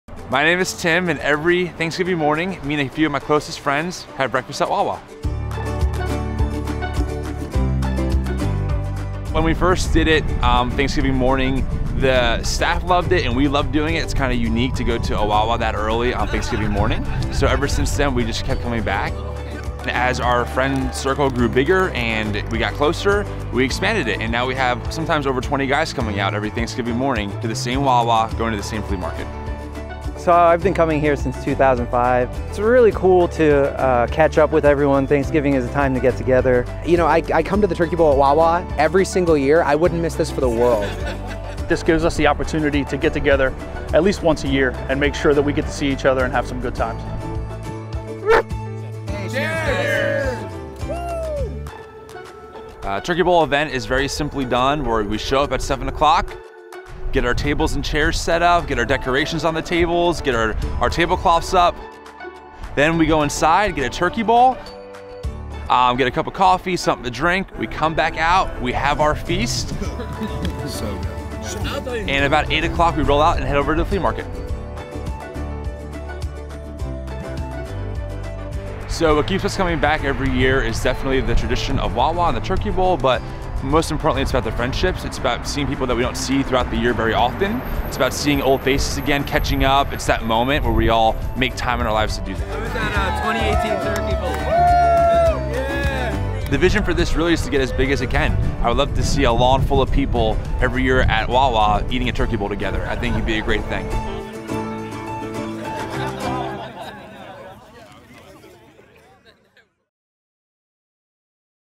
The Turkey Bowl Crew: A Wawa Friendsgiving
For 13 years, this big (and growing) group of friends have gathered at Wawa on Thanksgiving morning. Meet the TurkeyBowl crew.